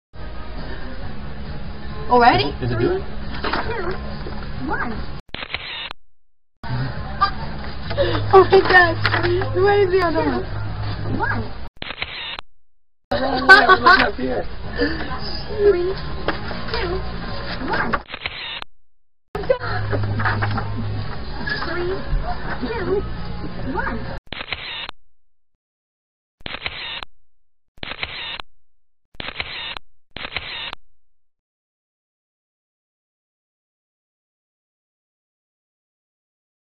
Save the date
Our 4th attempt at a photo booth movie. We are not very good lol